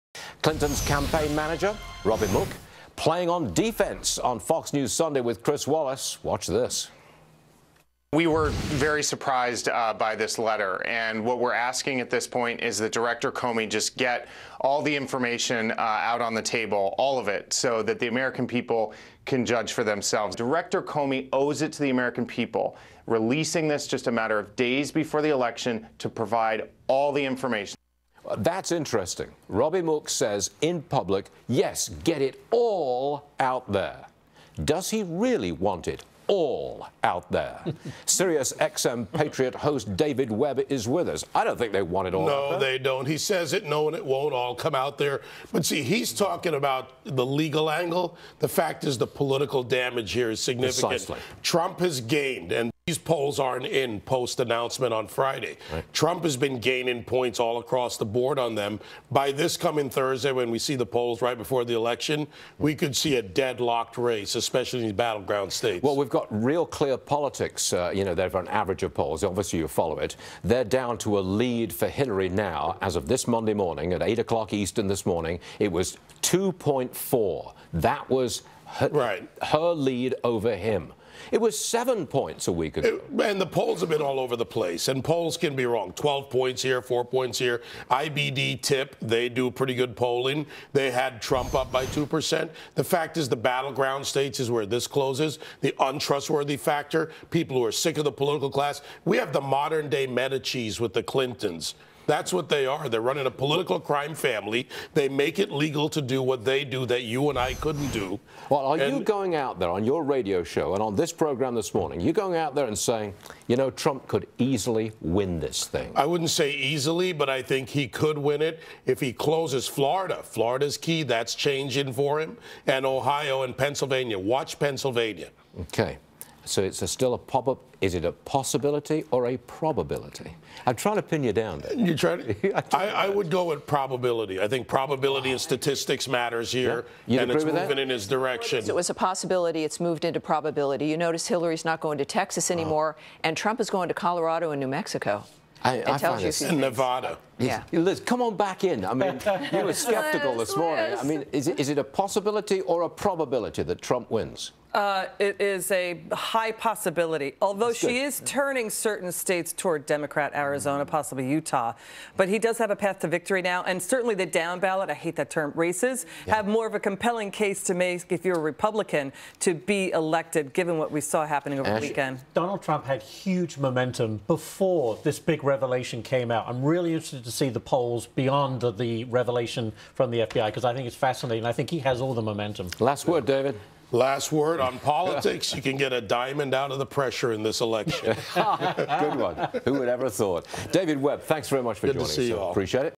Is Trump Winning The Election More of a Possibility or Probability? - David Webb
David Webb joins Stuart Varney to discuss his take on the impact of the FBI probe on the Hillary Clinton email scandal.